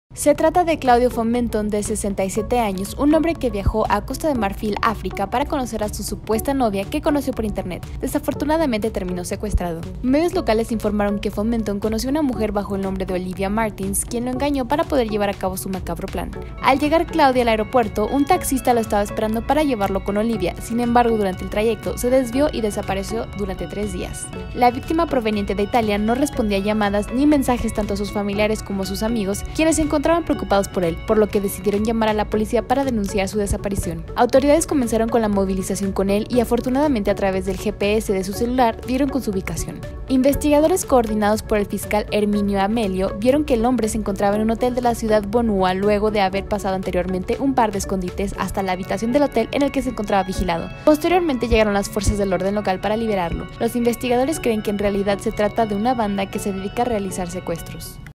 Hombre de la tercera edad viaja a África para conocer a su novia; termina secuestrado
Lo que parecía ser un encuentro amoroso con su novia a quien había conocido por internet, terminó siendo un macabro plan para secuestrarlo.